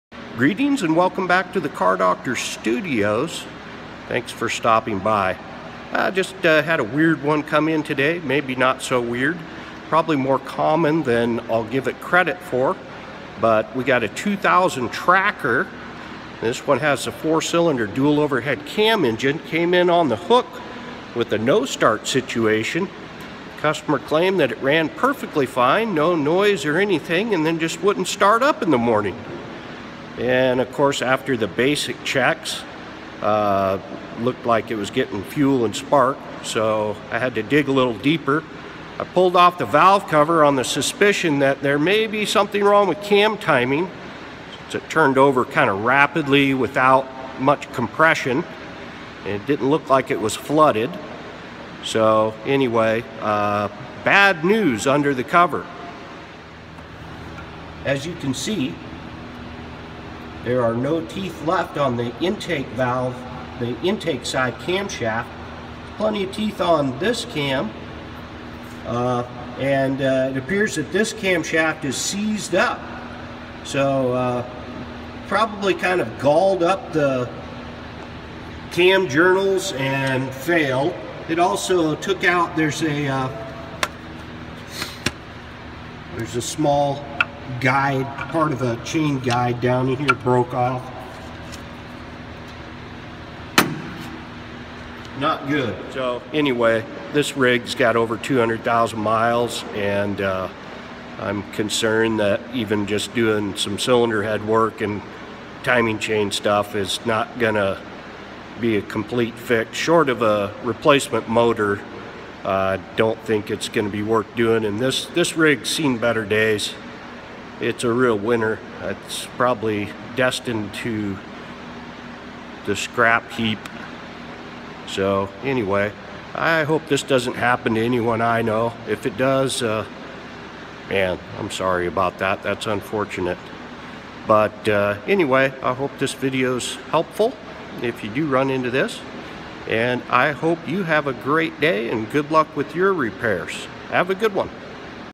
Chevy Tracker Won't Start - Chevy Tracker DOHC Camshaft Timing Chain Failure
In this video we look into a no start issue with a Chevy Tracker with the DOHC four cylinder engine. What we found was shocking.